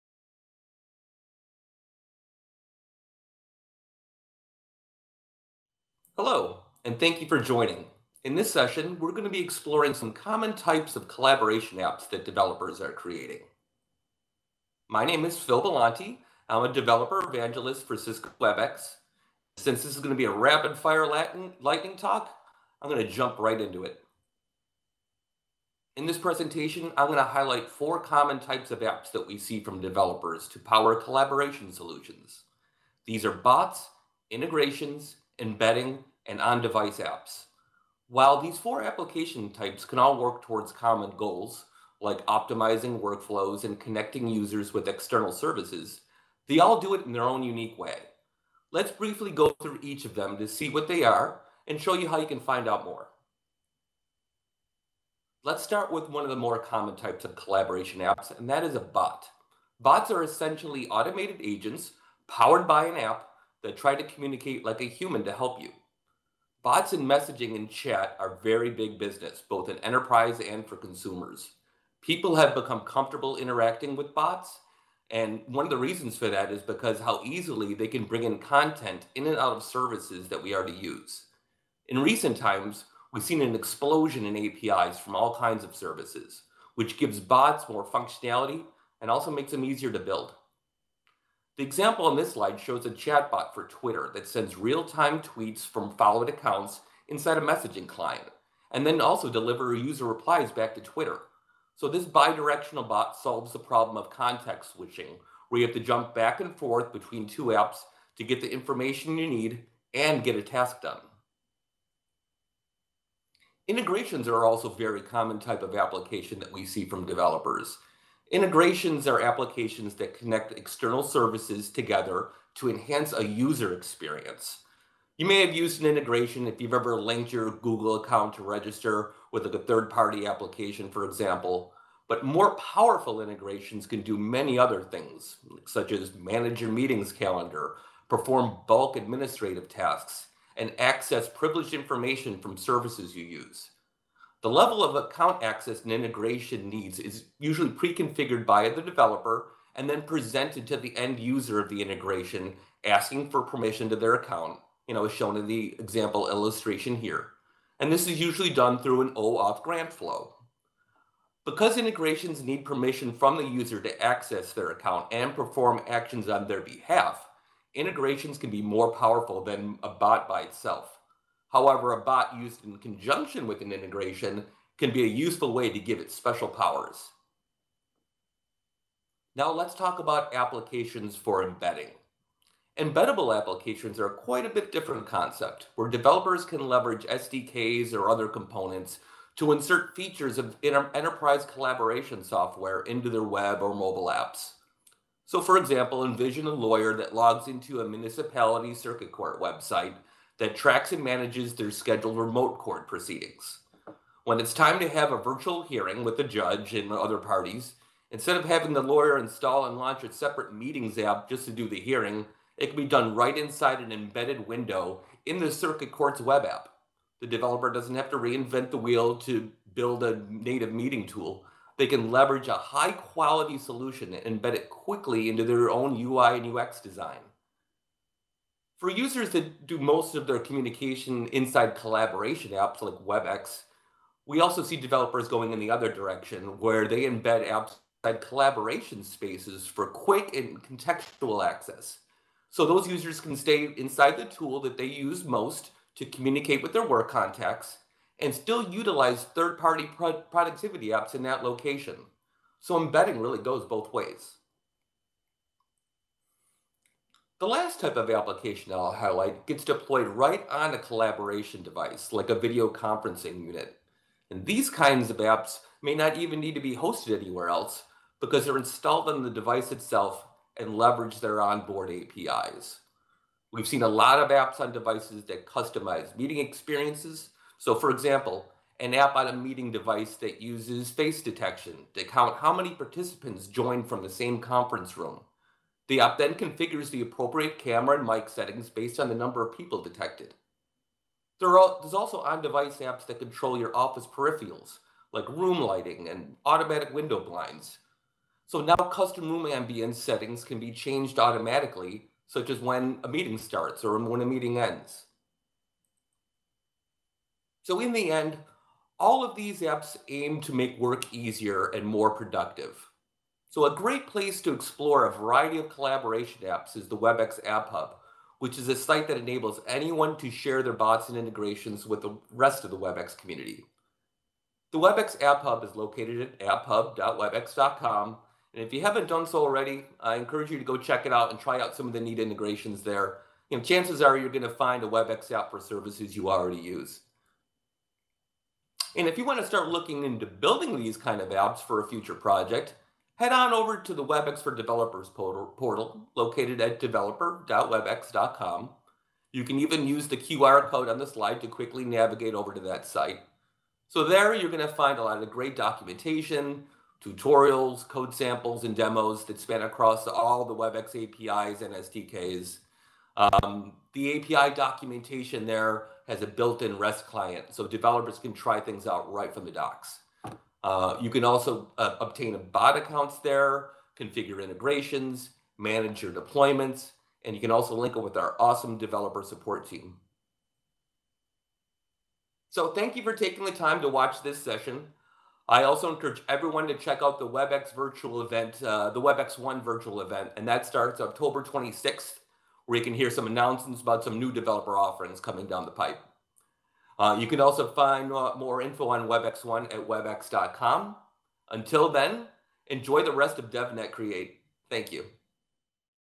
DevNet Create 2021 - Exploring different types of collaboration apps
Lightning Talk. This session will provide a brief overview of different collaboration applications and integrations that are being used today, for optimizing workflows and getting jobs done more efficiently. We will highlight some real-world examples for a variety of different verticals, along with some more general use cases to help you better envision how these kinds of apps can be utilized in your day-to-day tasks.